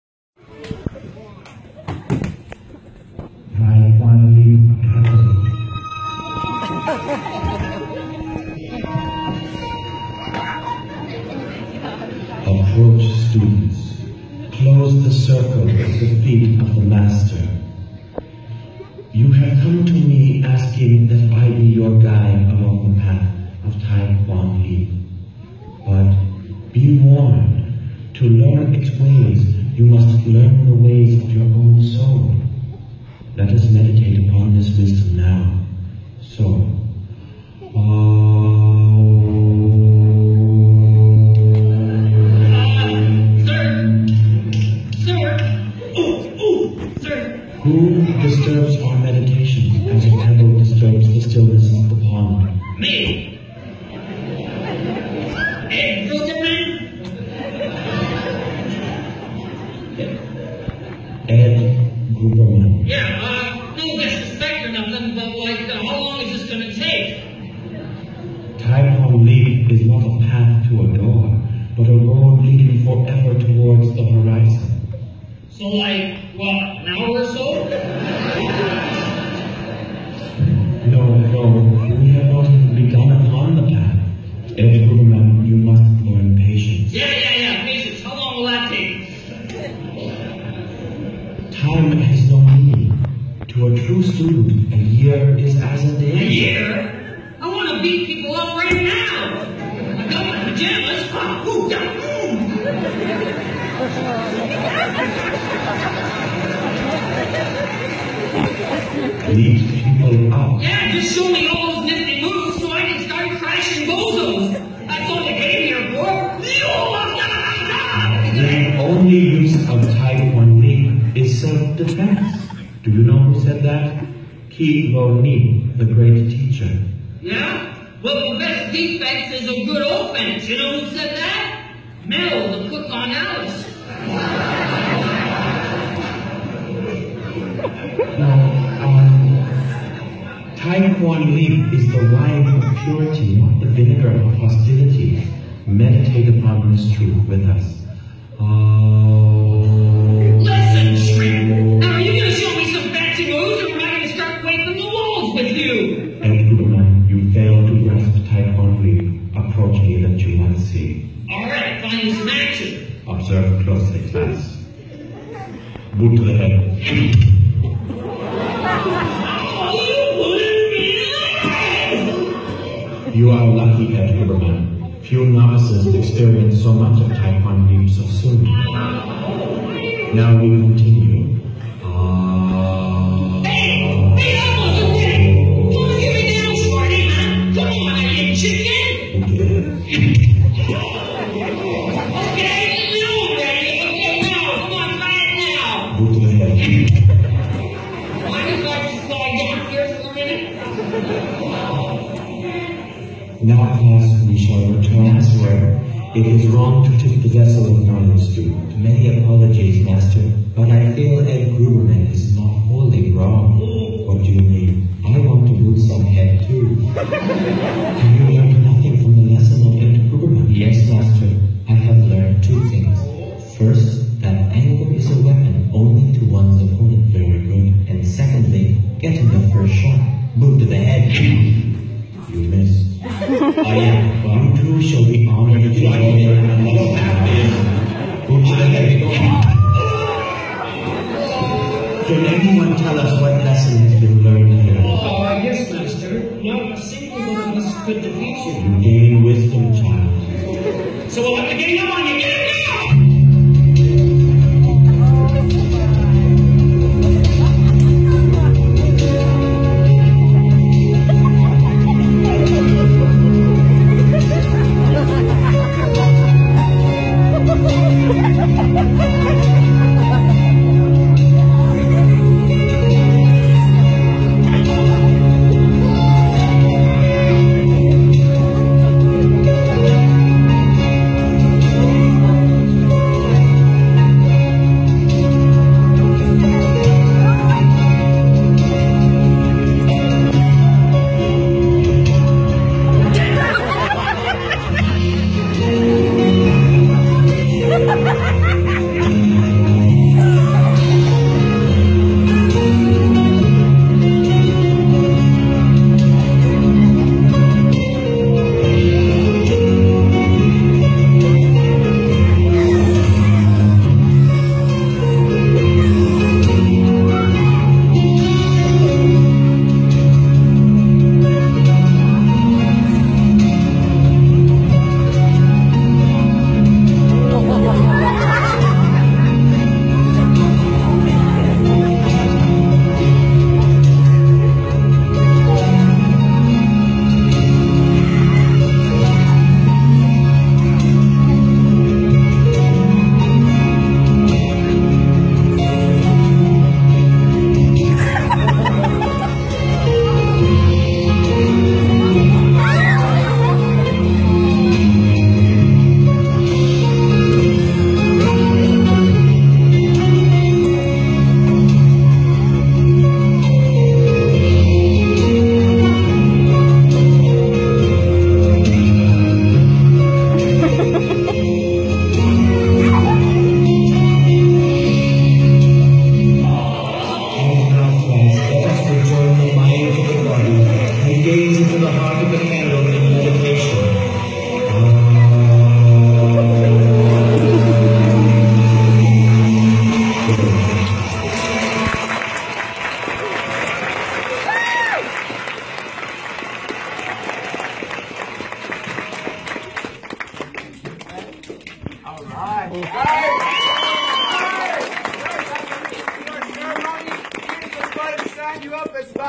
Boot to the Head Skit
Enjoy out video of the Boot to the Head skit performed at our most recent belt  graduation ceremony!